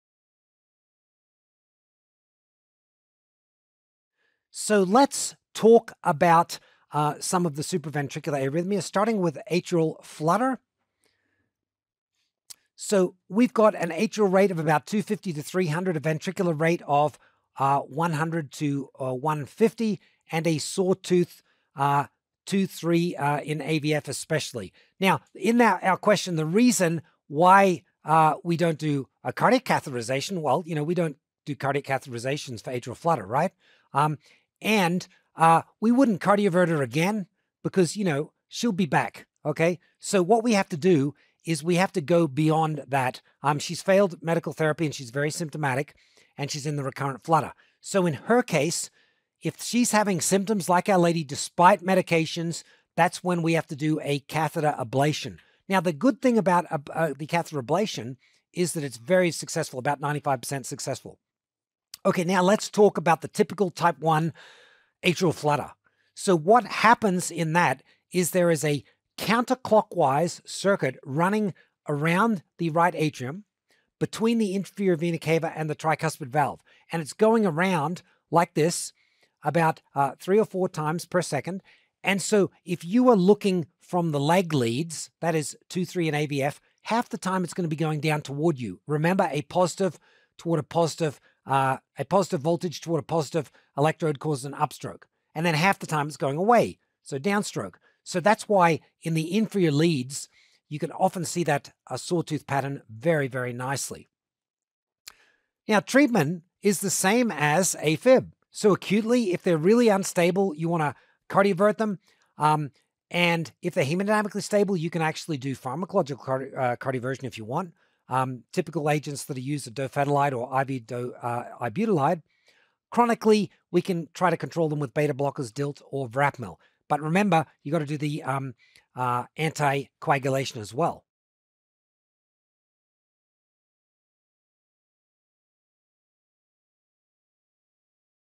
Atrial Flutter | ABIM Board Review
This snippet is from Dr. John P. Higgins in the Cardiology lecture of the MedStudy’s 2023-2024 Internal Medicine Video Board Review.

Atrial Flutter
 ABIM Review from the syllabus
• Atria 250–300 bpm; vent. 100–150 bpm; “Sawtooth” II,
III, aVF

• Typical/Type I = cavotricuspid isthmus (CTI)-dependent
counterclockwise running between IVC and
tricuspid valve
• Vagal maneuvers/AV nodal blockers — slow ventricular
response; often have structural heart disease or COPD
• Rx = same as AFib! Acute: DC cardioversion if unstable;
 Pharm cardioversion in hemodynamically stable:
dofetilide or IV ibutilide; Chronic: BB, diltiazem, or
verapamil

• Symptoms despite meds: Catheter ablation (95%
success)